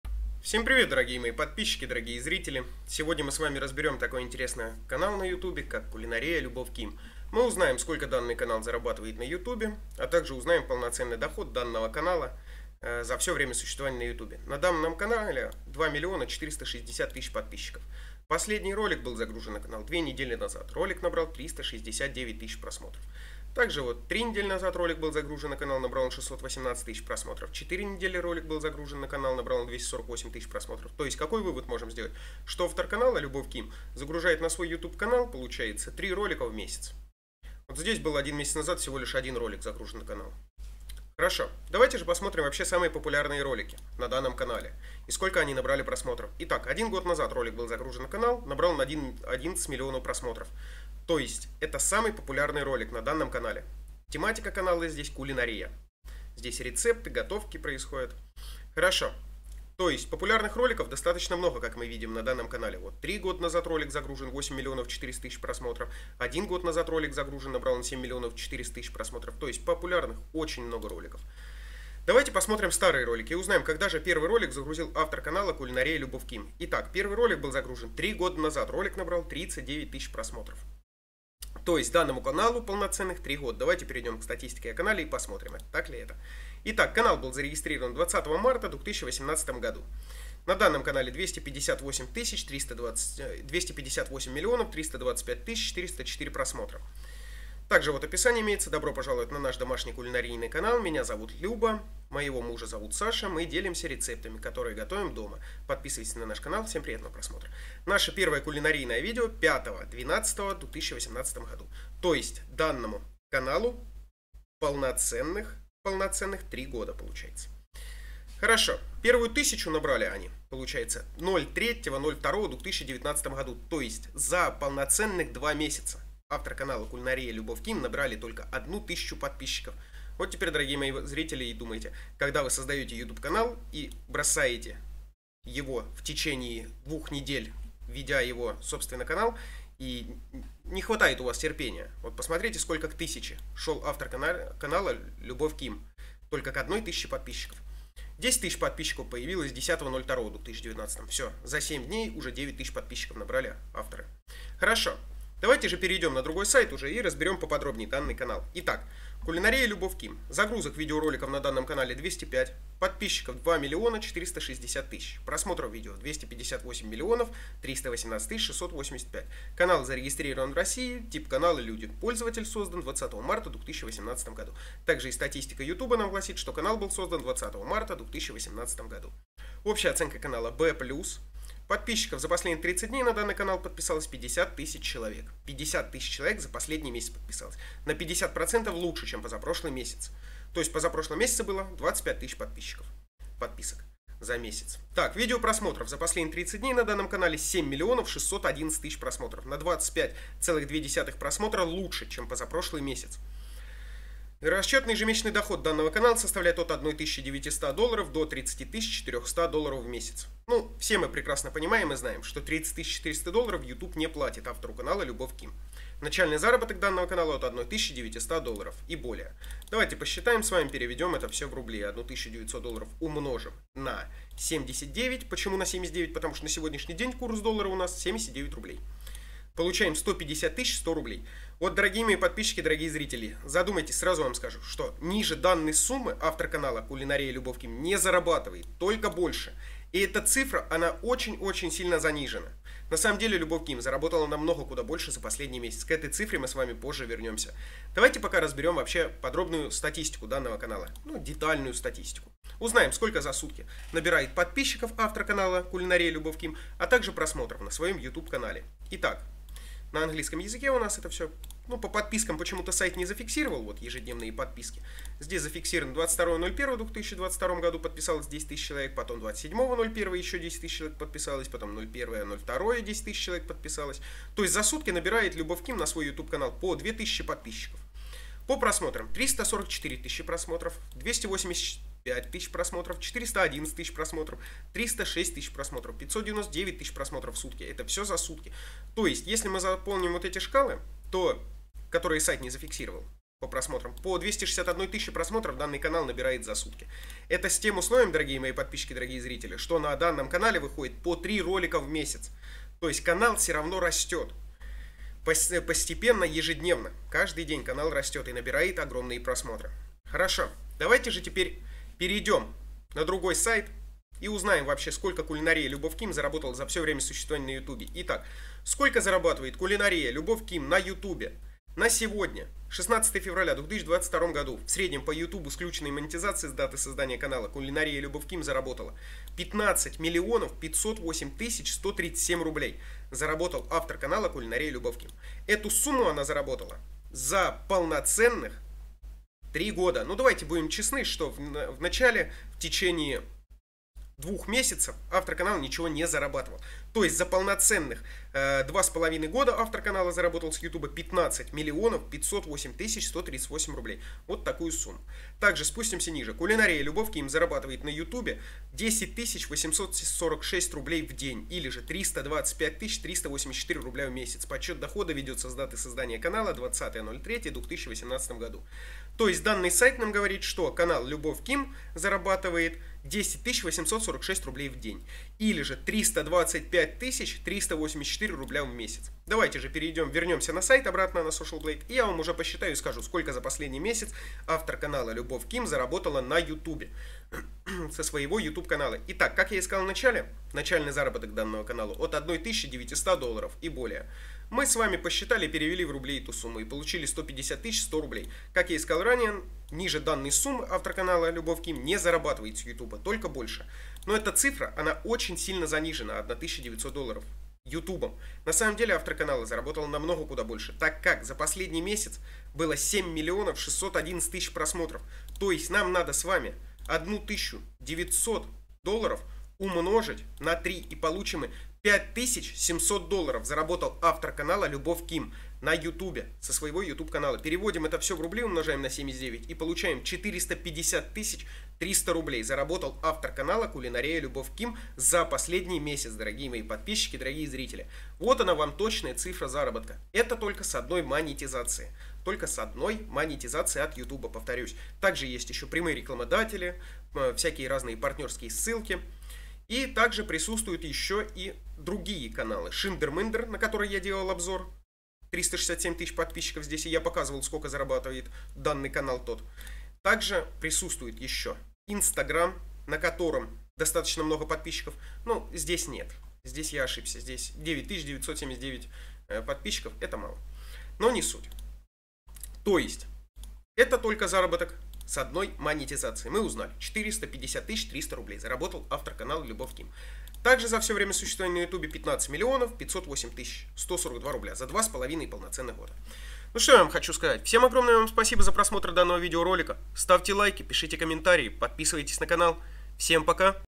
Сколько зарабатывает КУЛИНАРИЯ Любовь Ким на Youtube
Всем привет,в этом видео я расскажу Сколько зарабатывает КУЛИНАРИЯ Любовь Ким на Youtube,так же узнаем полноценный доход канала КУЛИНАРИЯ Любовь Ким за всё время существования на Youtube,проведем полноценный обзор канала КУЛИНАРИЯ Любовь Ким,узнаем статистику а так же заработок канала КУЛИНАРИЯ Любовь Ким.Приятного просмотра